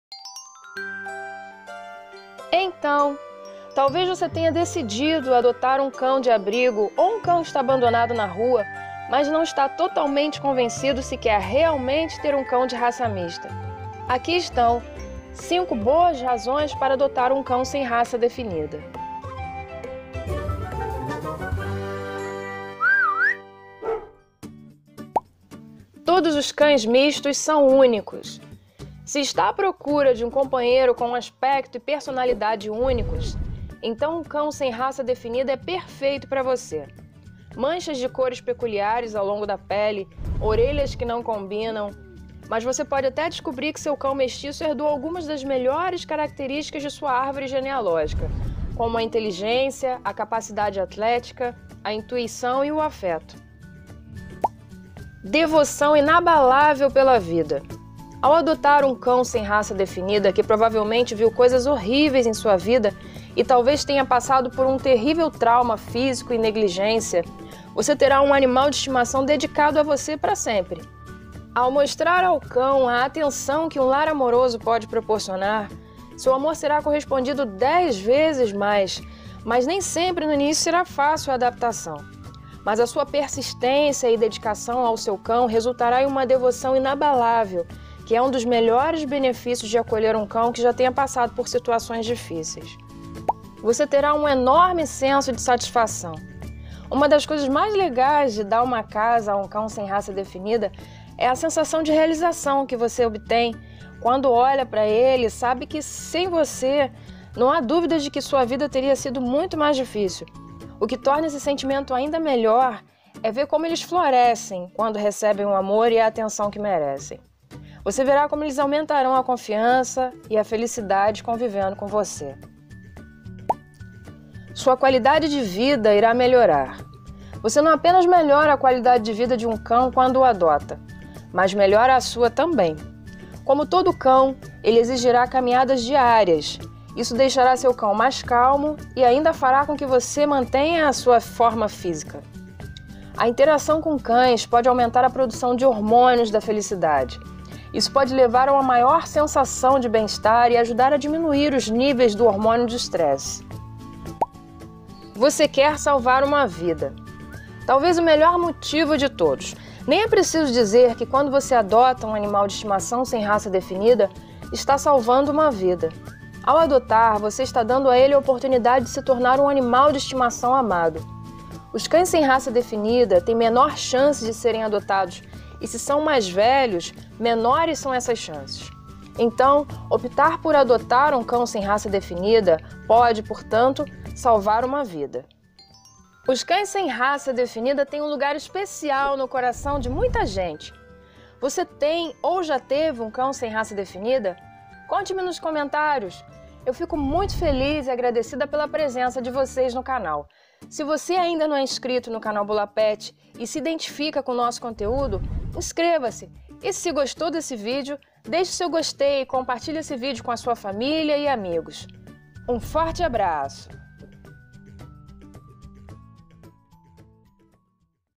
Motivos pouco conhecidos para adotar um cão SRD - cachorro de raça mista
Adotar um cão é uma experiência única. Já pensou em adotar um cão de raça mista? Sem dúvida, terá um efeito positivo e profundo em sua vida. Aqui estão cinco boas razões para adotar um cão sem raça definida.